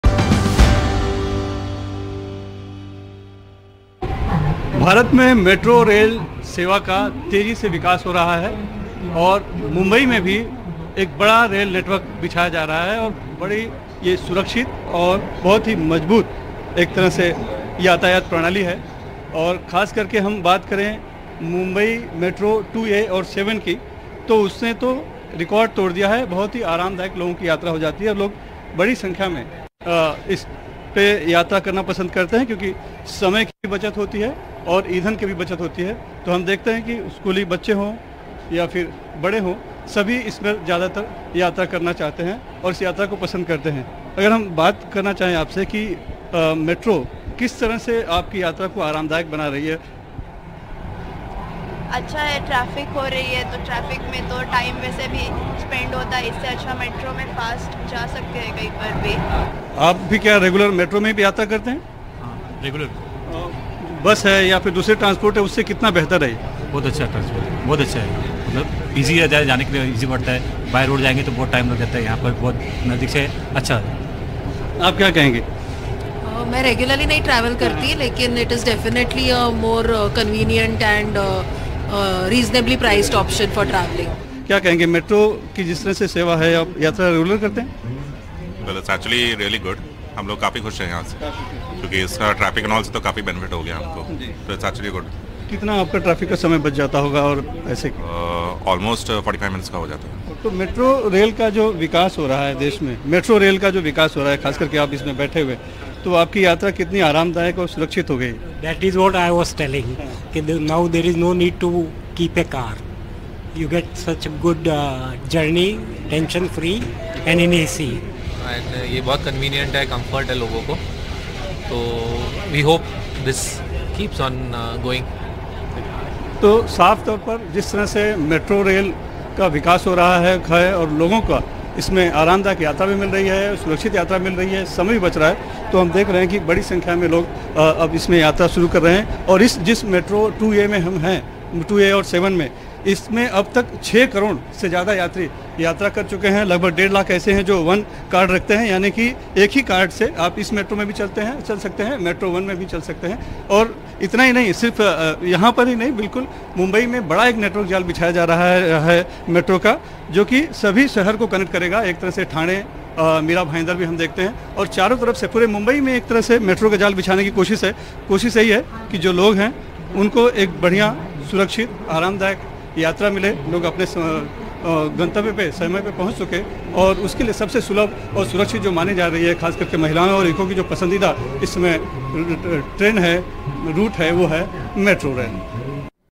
Mumbai Metro Rail: Bid To Decongest City Roads
The Mumbai Metro reduces traffic congestion in the city and supplements the overcrowded suburban rail network. It has been built in three phases over 15 years and is expected to be completed by October 2026. The Mumbai Metro is the sixth longest operational metro network in India with an operational length of 46.4 km. When completed, it will have 14 high-capacity metro rail lines and one metrolite line over 356 km with 24% of it underground and 286 stations.